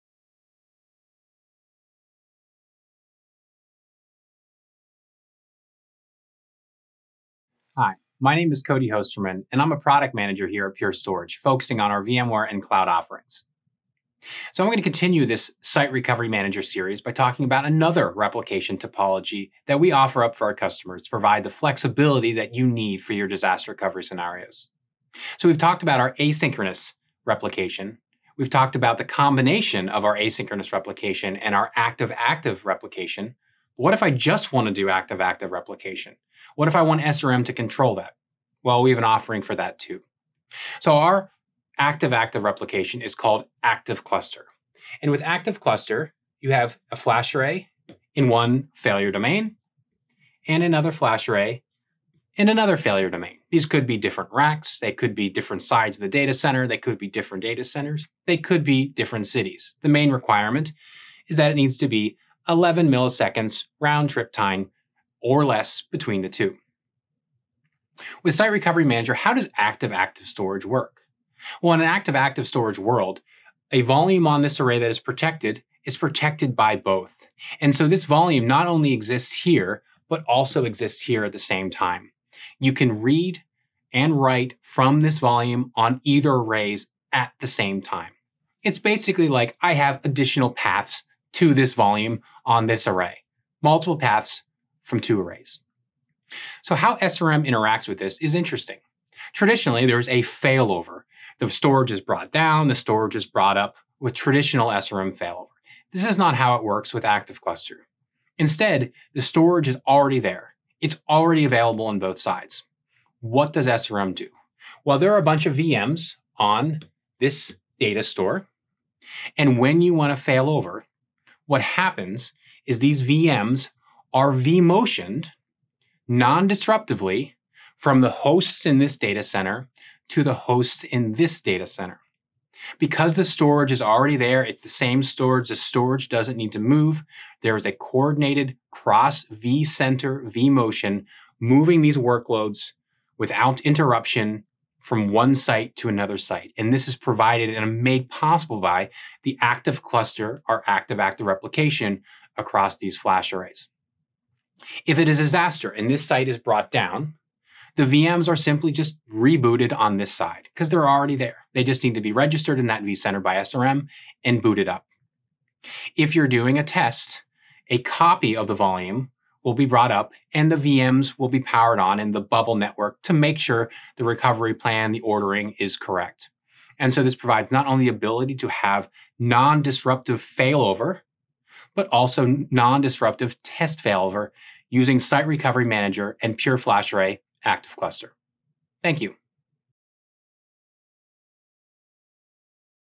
SRM and Stretched Storage
Looking to take your Disaster Recovery plan to next level? In this video, watch Cody talk about Stretched Storage with Site Recovery Manager.